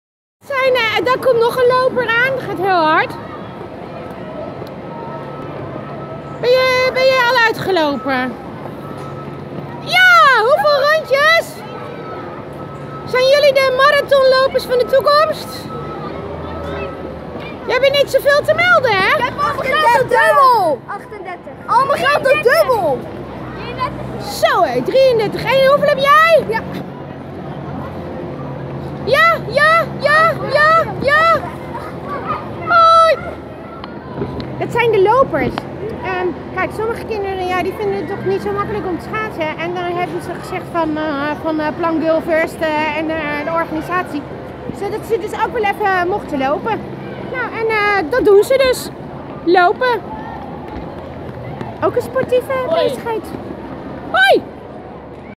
Hardlopers op de binnenbaan..ook zeer sportief
Brechje bij Schaatsen voor Water ,Thialf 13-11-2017 middagploev